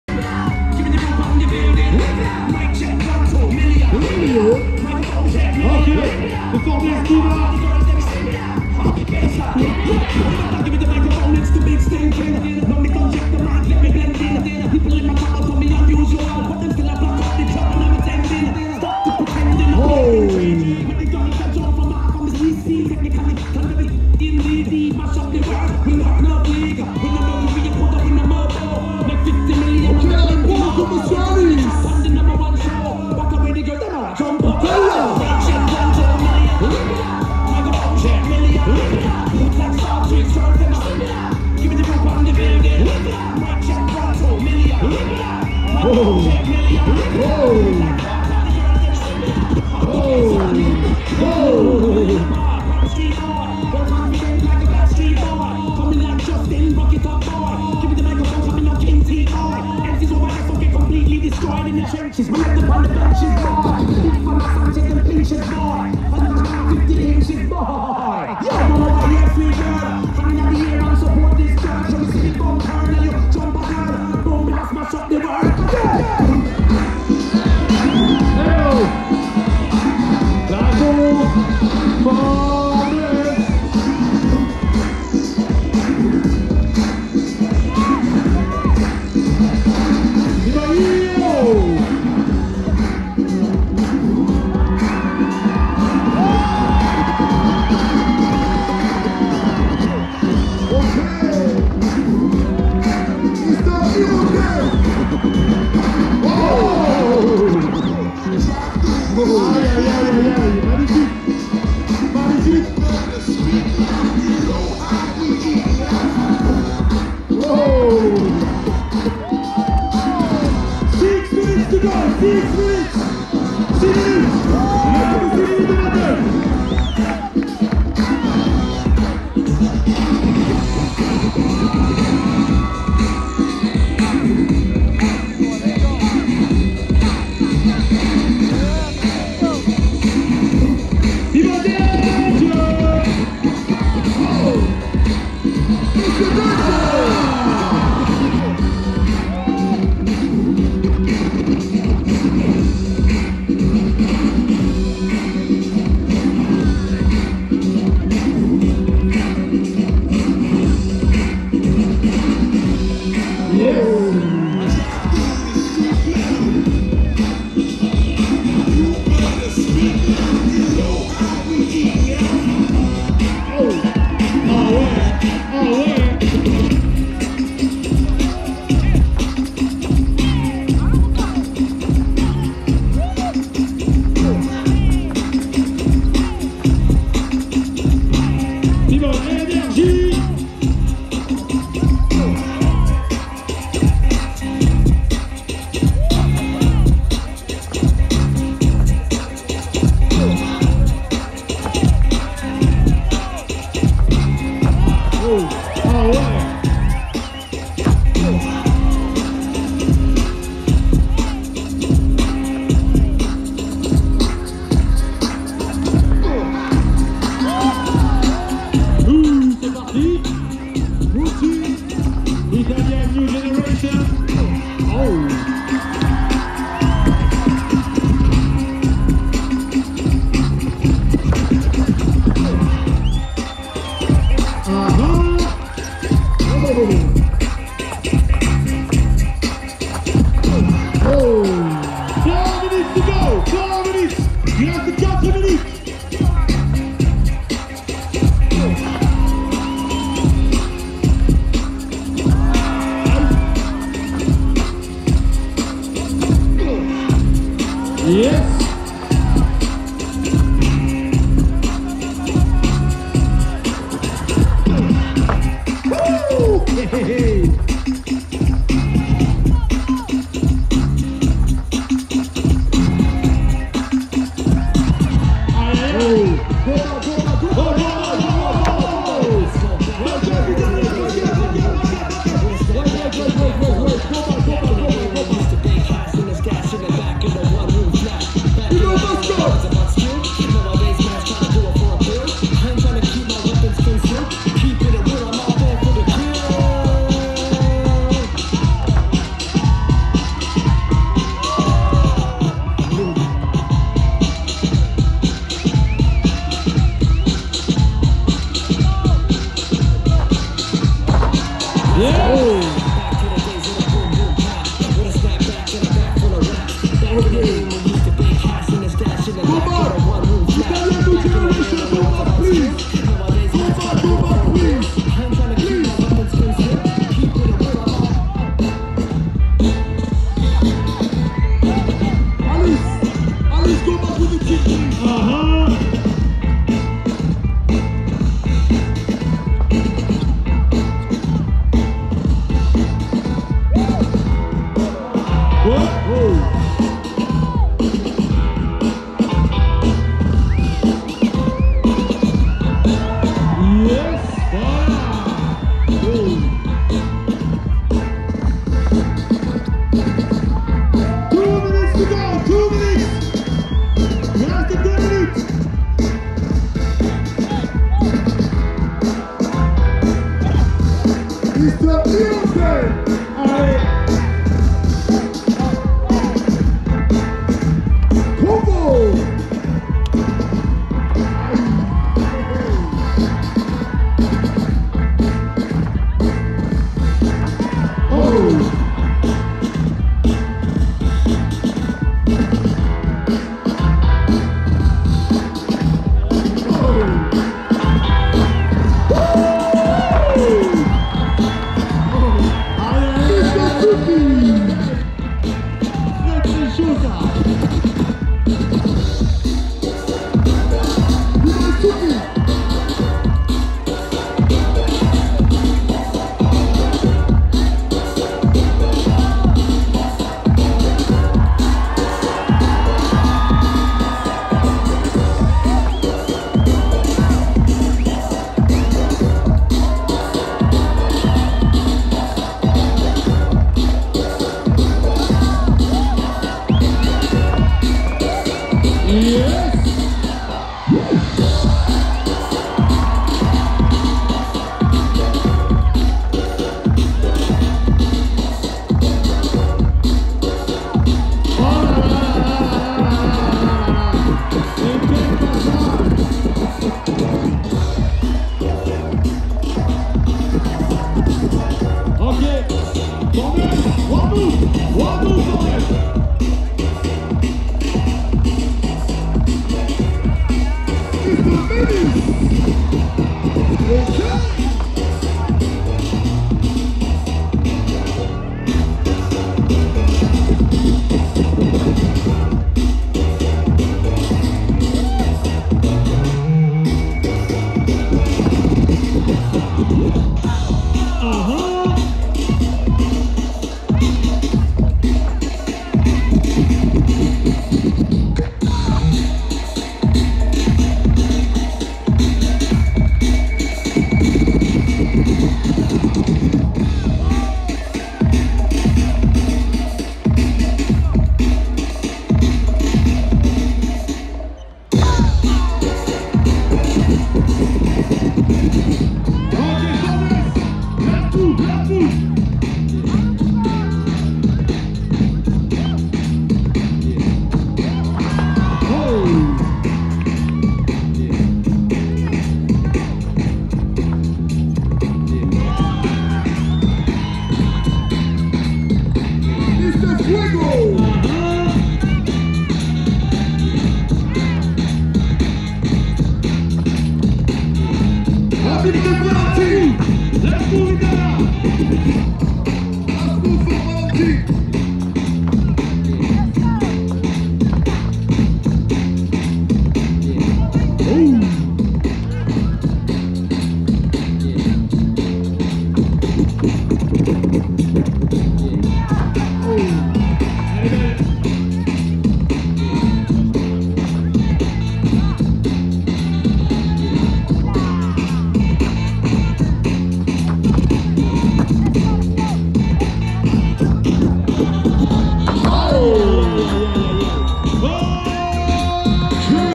Italian New Generation (Italy) VS Formless Crew International
Event: Block Party 2022
City: Perpignan
Organisation: La Casa Musicale
Organisation contact Follow and Support Below!!

✪ BBoy World Entertainment | Since 1999